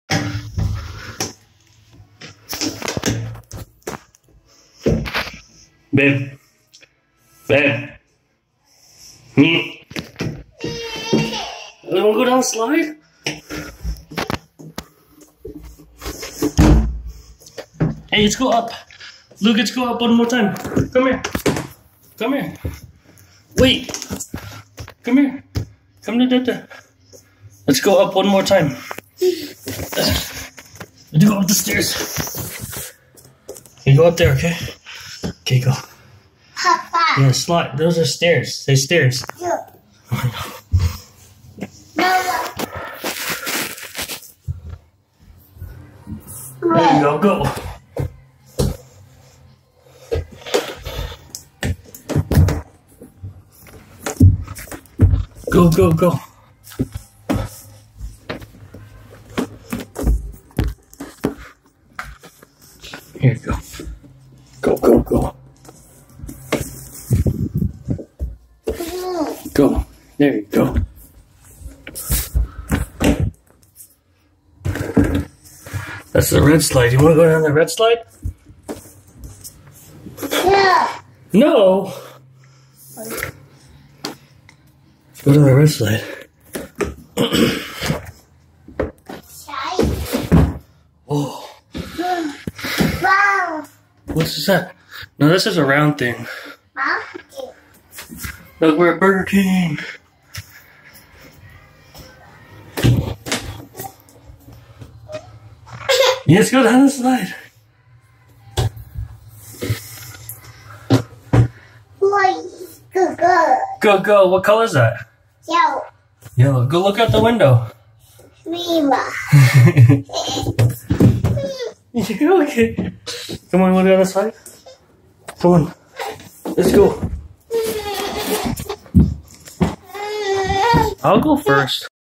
Luke doesnt like the big indoor toy
Luke not happy about the big toy at burger king